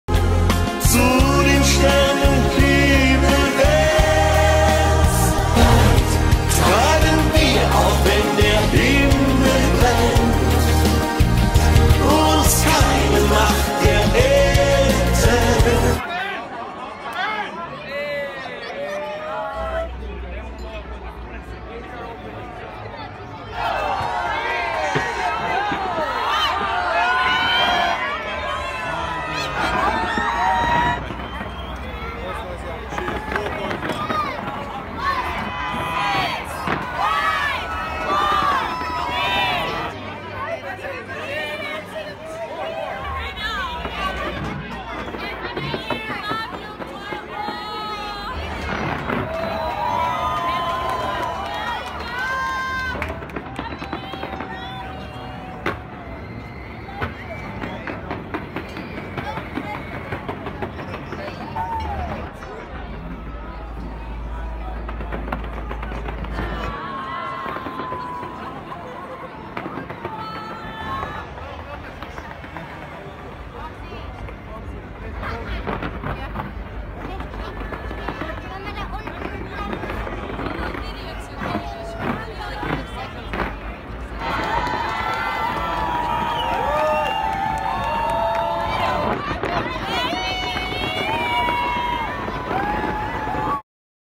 Dubai 2020 am Burj Khalifa
Es ist schon ein Erlebnis Silvester in Dubai direkt am Burj Khalifa zu feiern!
Für alle die das einmal erleben möchte ist es wichtig sich früh genug eine Eintrittskarte zu kaufen!
Denn sonst hat man keine Chance dicht dabei zu sein!